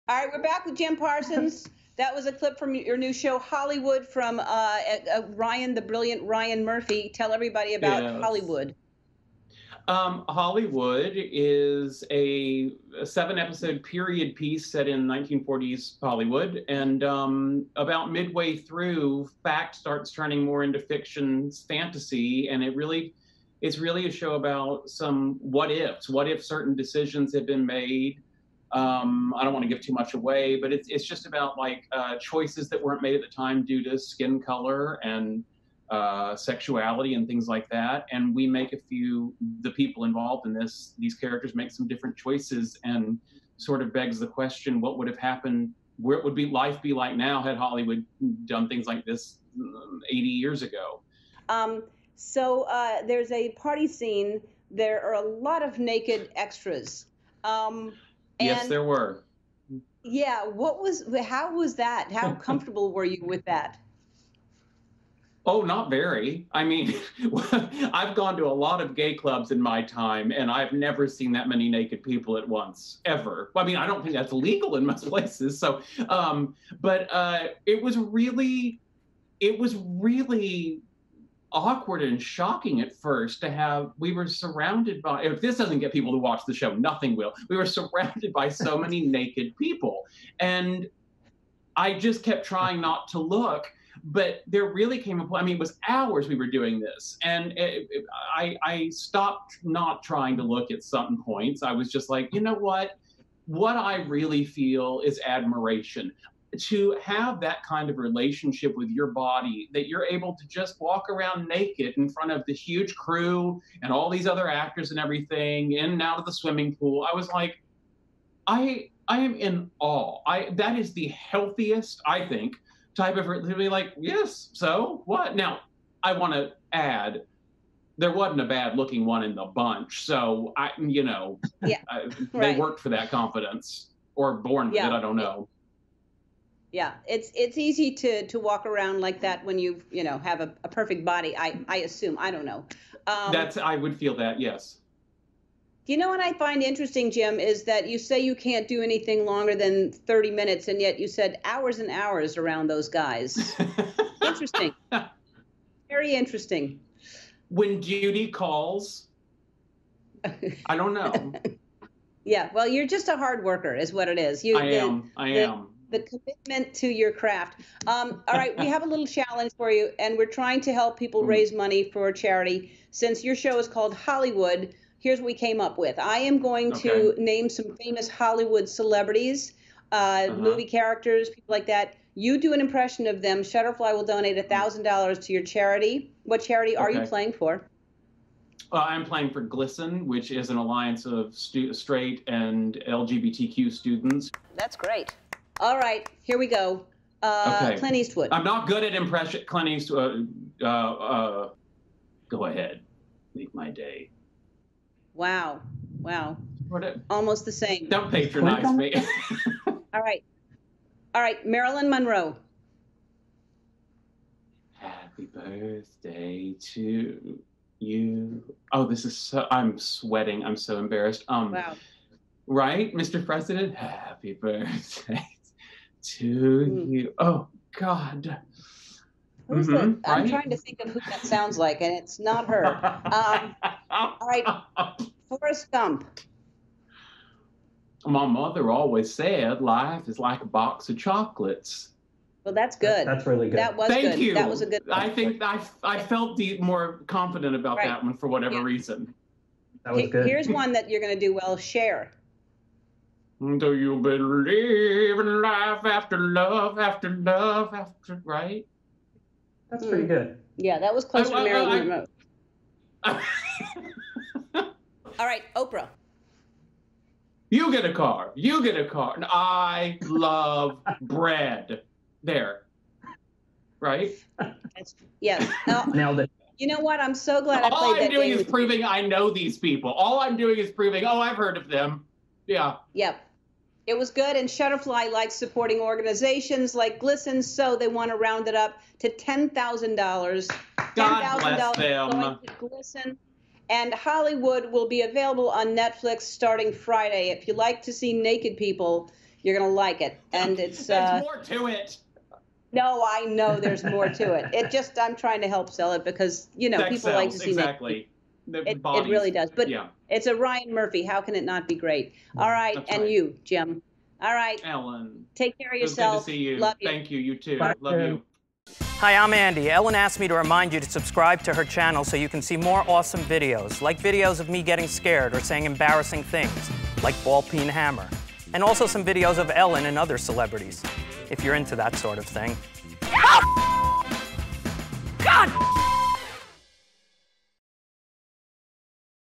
Naked People Surrounded Jim Parsons on the ‘Hollywood’ Set
Jim Parsons chatted with Ellen about his new Netflix miniseries “Hollywood,” for which he filmed a scene while surrounded by naked extras. He explained how he’s never seen that many nude people at once and spent hours trying to not look at them. Plus, by doing impressions of iconic Hollywood stars and characters, the Emmy winner raised money for LGBTQ-focused organization GLSEN, thanks to Shutterfly.